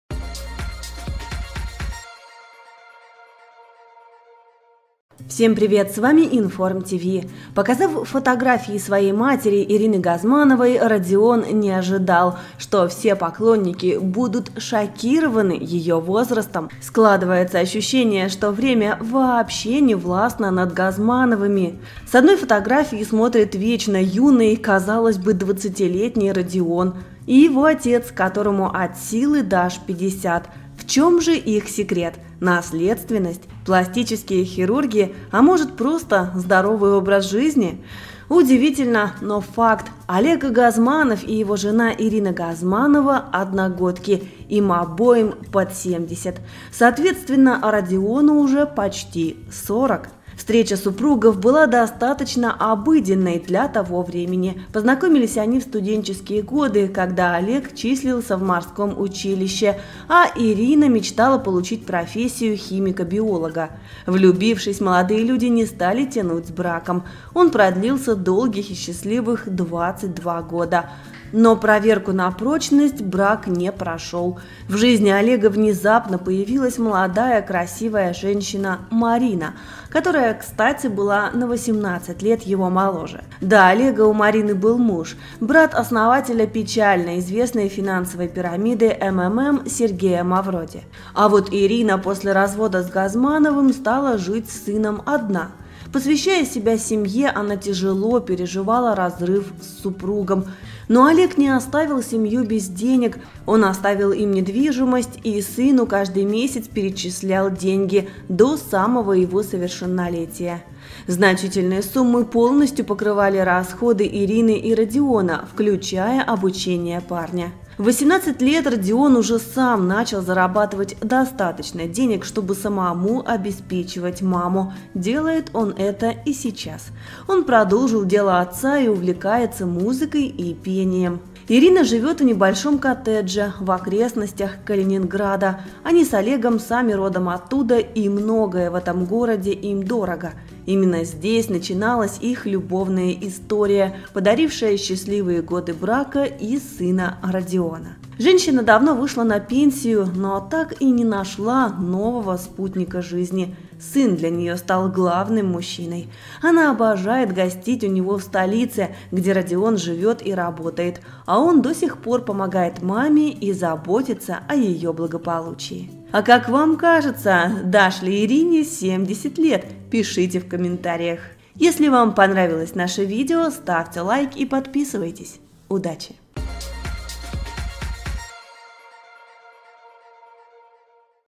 РАЗВОД БЫЛ УДАРОМ | Как живет 69-летняя Ирина Газманова, первая жена артиста и мама его сына
Показав фотографии своей матери Ирины Газмановой, Родион не ожидал, что все будут удивлены её возрастом.
Складывается ощущение, что время не властно над четой Газмановых. С фото смотрит вечно юный 20-летний Родион, и отец, которому от силы дашь 50. В чем же их секрет?